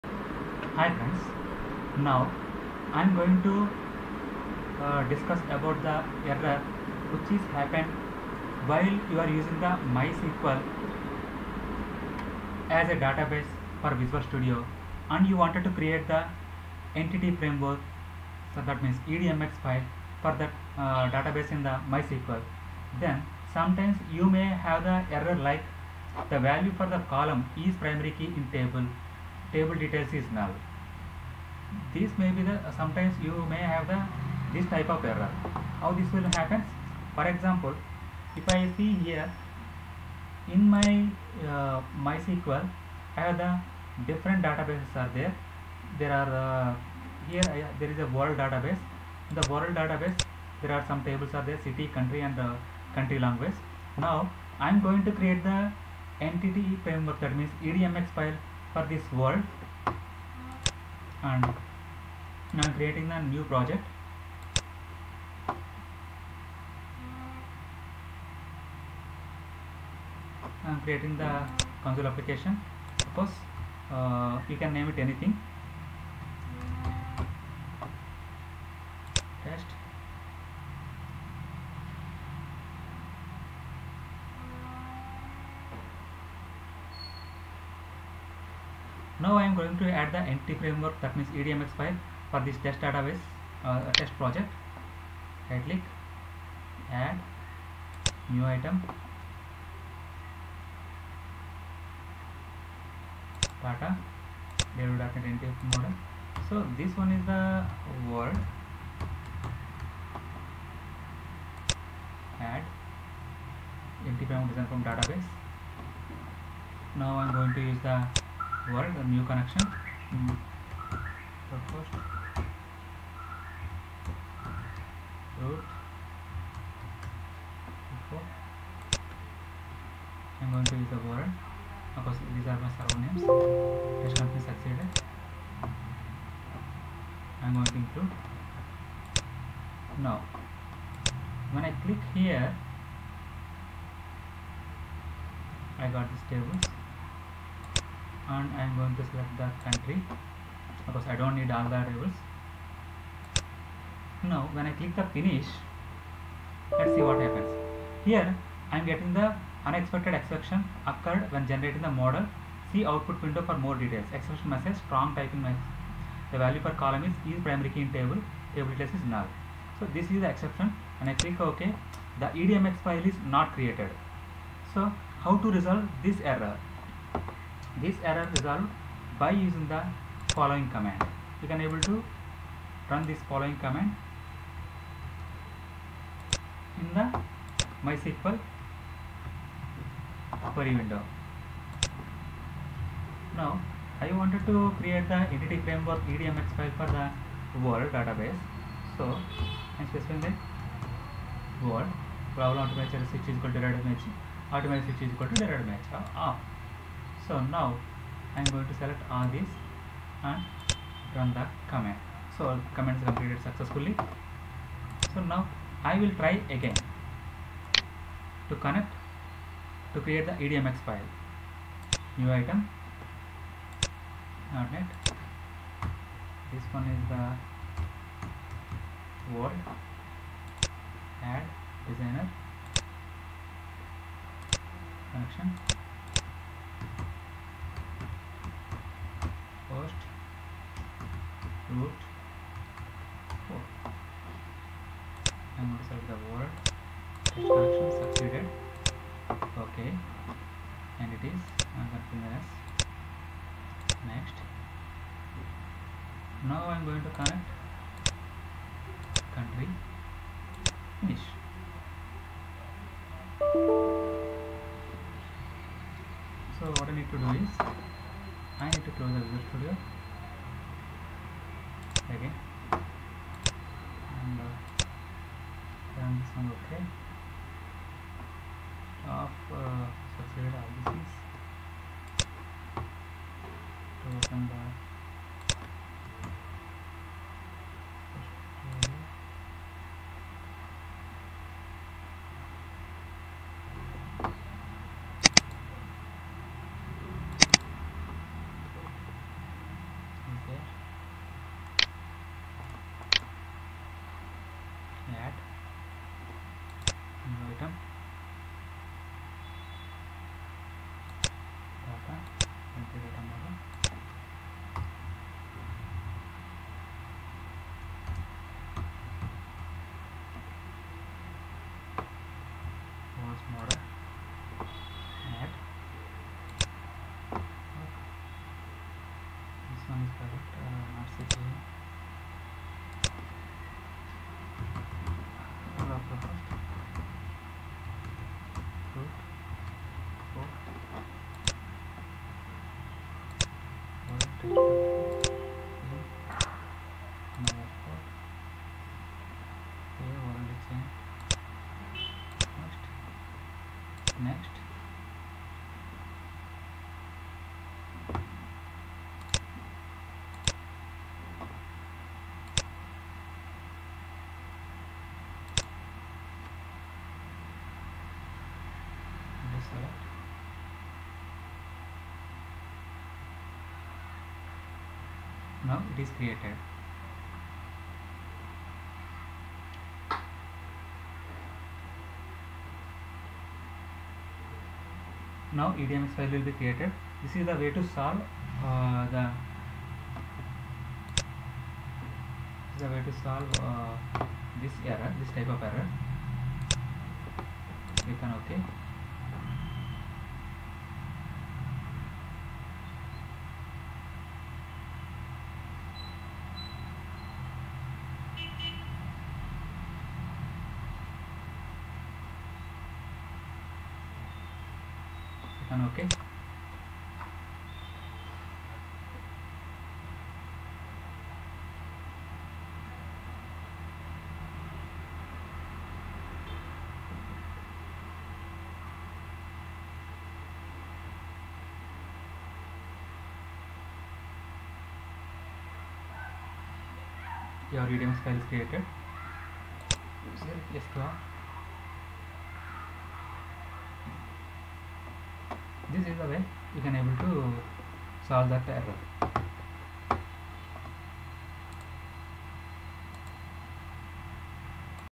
The value for column 'IsPrimaryKey' in table 'TableDetails' is DBNull
MySQL Entityframework Model creation error in visual studio